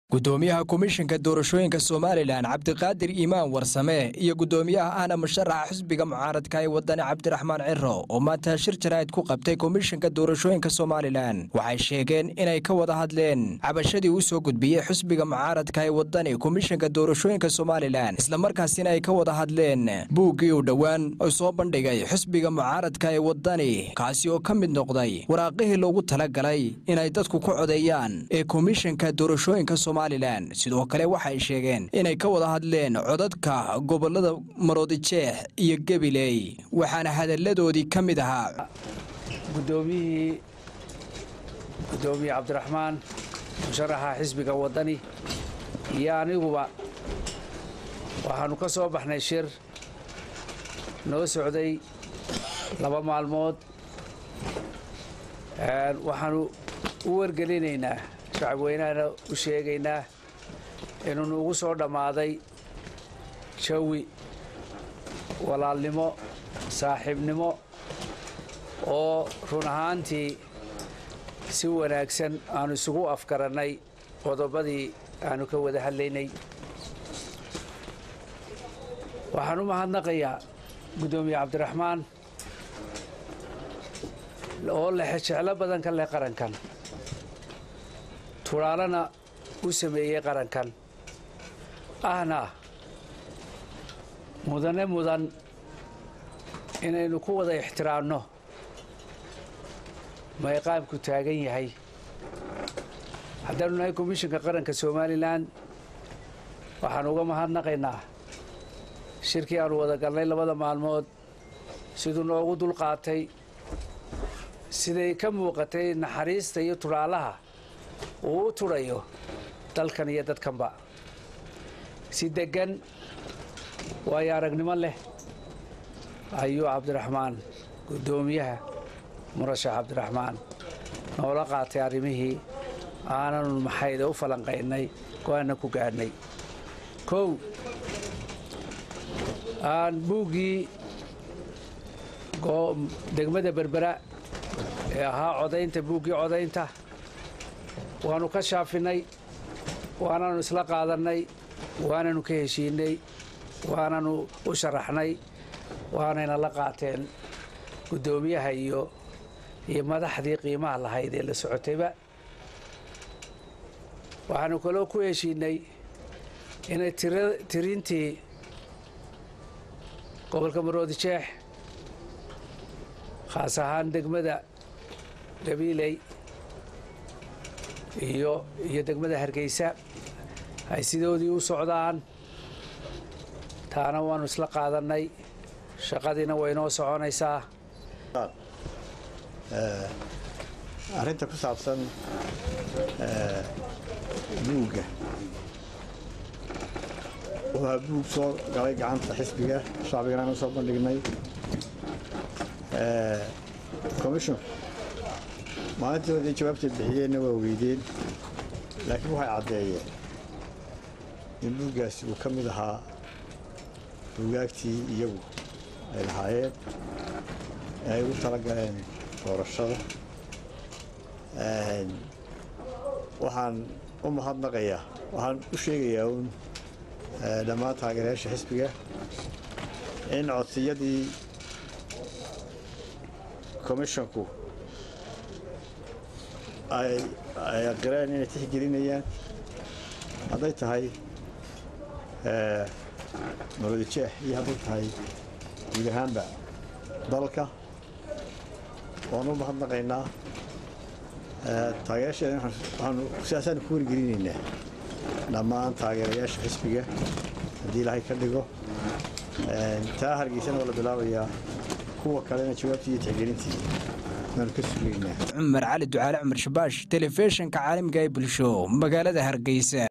DEG DEG XISBIGA WADANI IYO GUDIGA KOMISHINKA OO SHIR JARAAID QABTAY OO SHEEGAY INAY HISHIIS KAGADHI
HALKAAN KADAAWO WARAR KU SALAYSAN XAQIIQDA